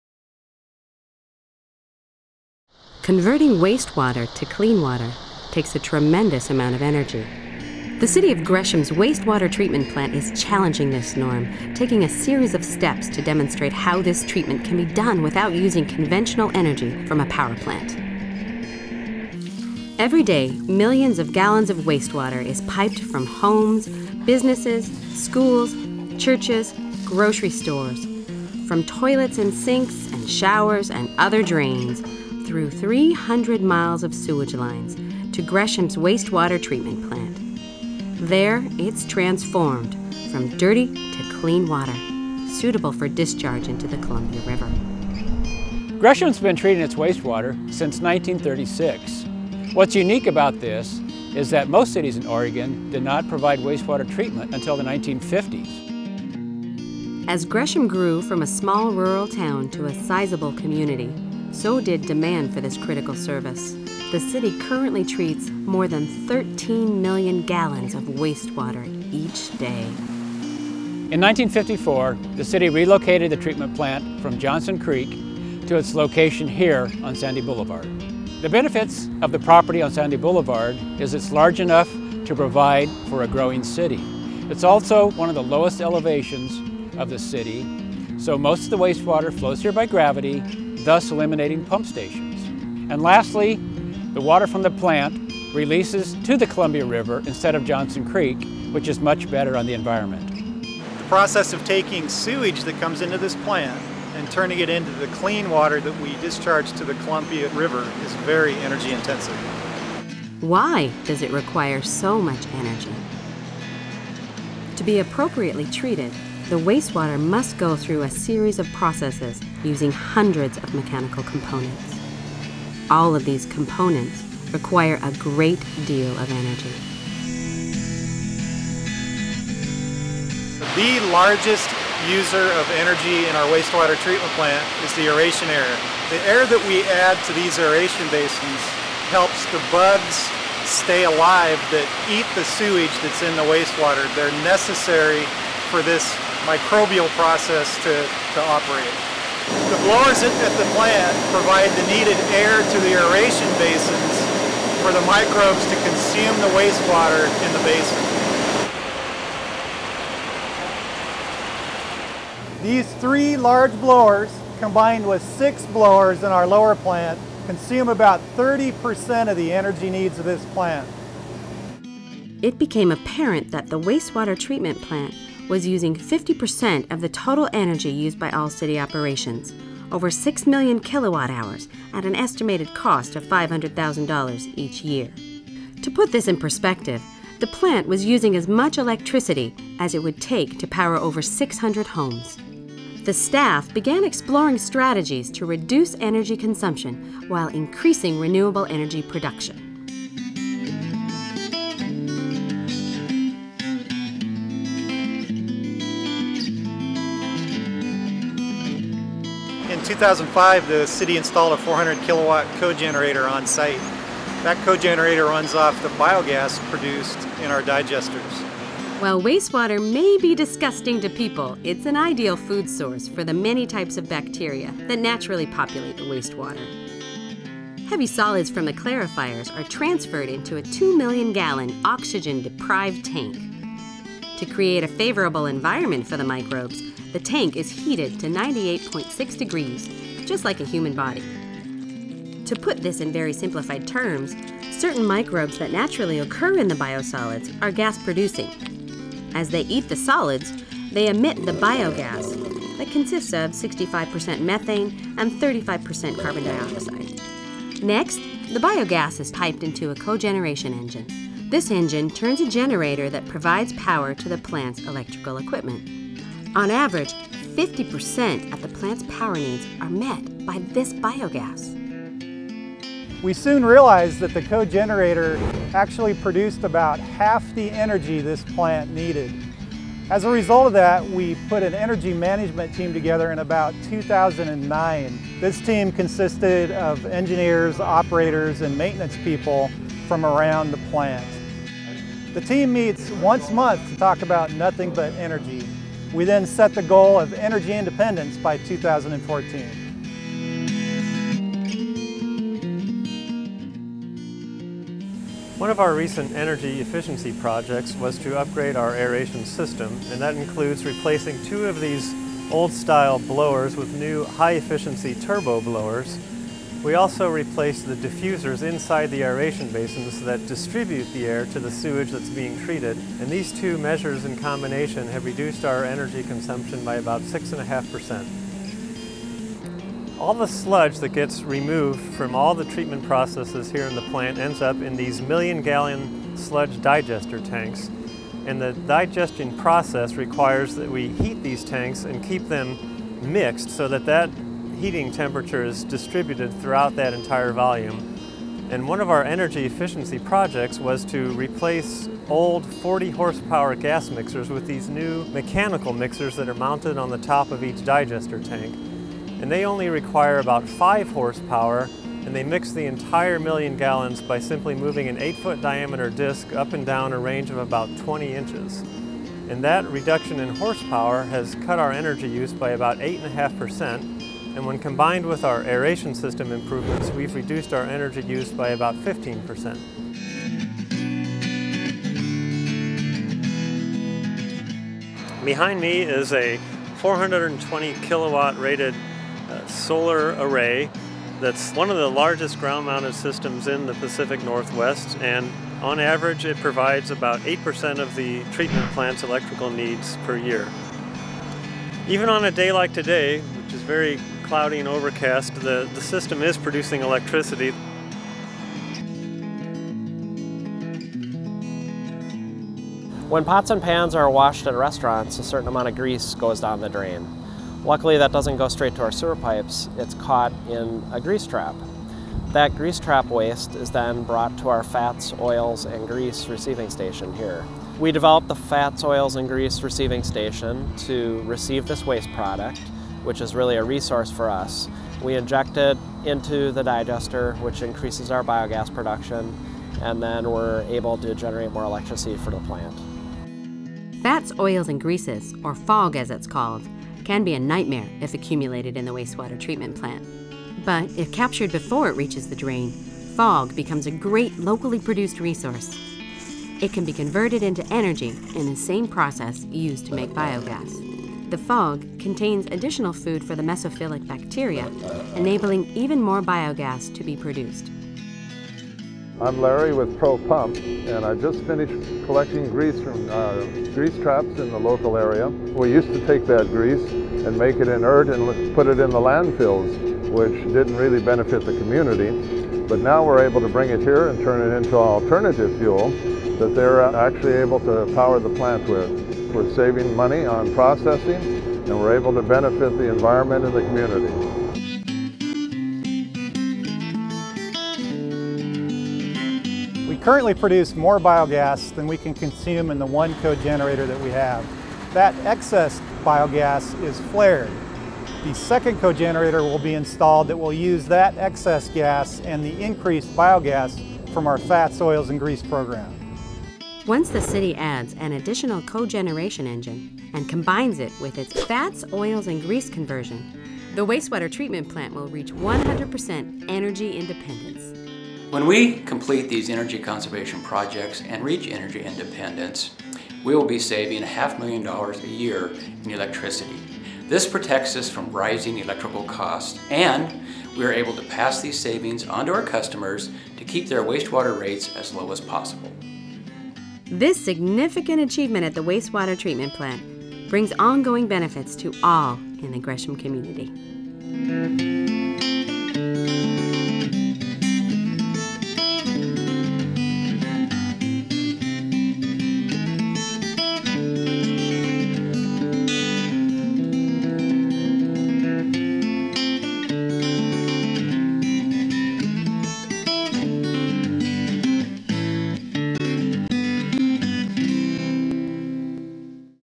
City of Gresham Wastewater Services: Clean Water From Clean Energy
The City of Gresham, Oregon, has an ambitious goal to makes its wastewater treatment plant energy independent by 2014, by producing its electrical power on site through a combination of cogeneration and solar power. This video shows what City staff have accomplished so far and what remains to be done.